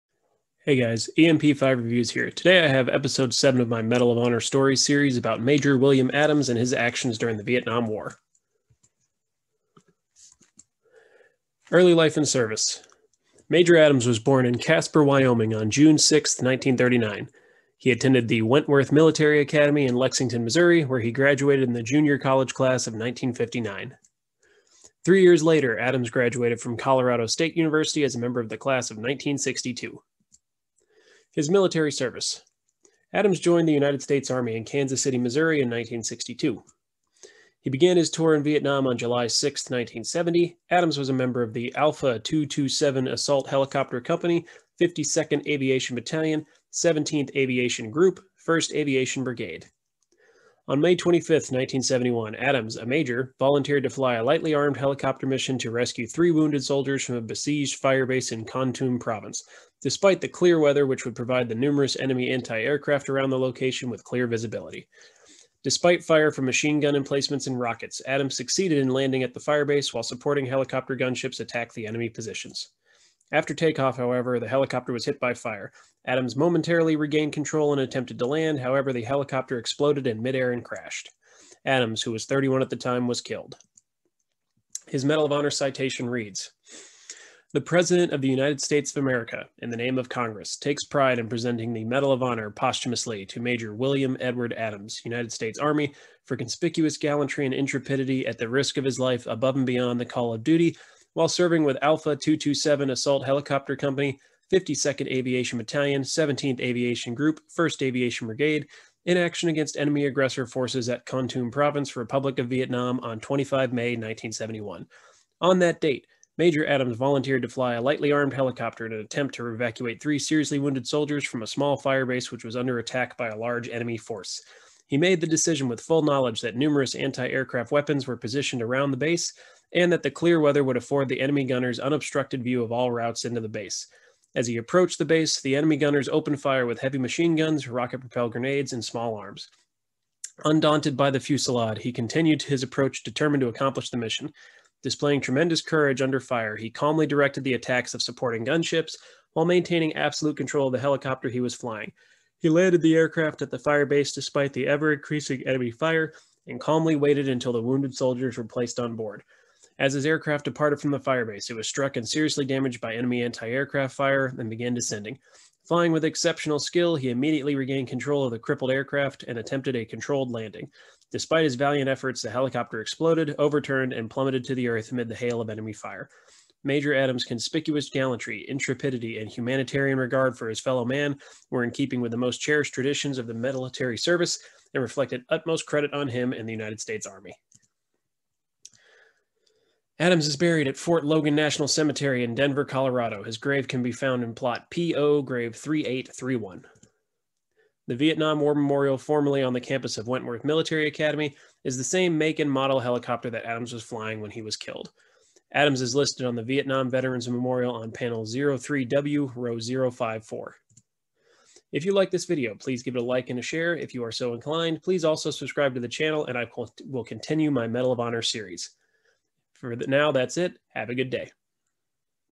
Medal of Honor Stories Episode 7: MAJ William Adams (Vietnam War)
This is episode 7 in my Medal of Honor Stories series. I will be going through Medal of Honor winners in alphabetical order in order to summarize what caused them to receive the Medal of Honor.